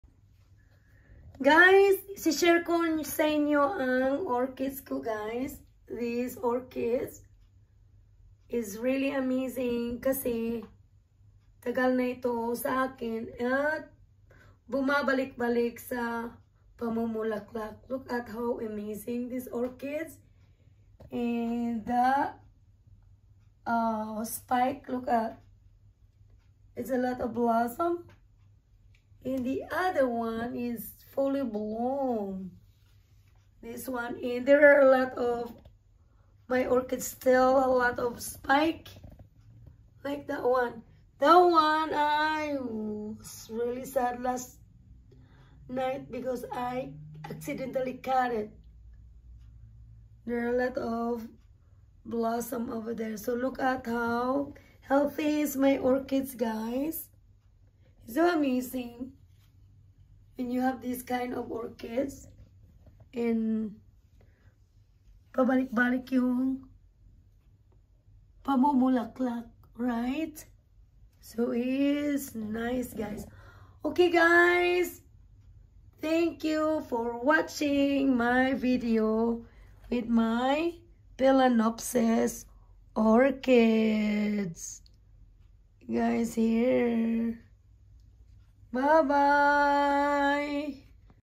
SUMMER GROWTH PHALAENOPSIS ORCHIDS
houseplant orchids# its amazing to have this orchids. If you notice it really healthy and lot of flowers soon. There are some fully blooms but some still shows up the spike.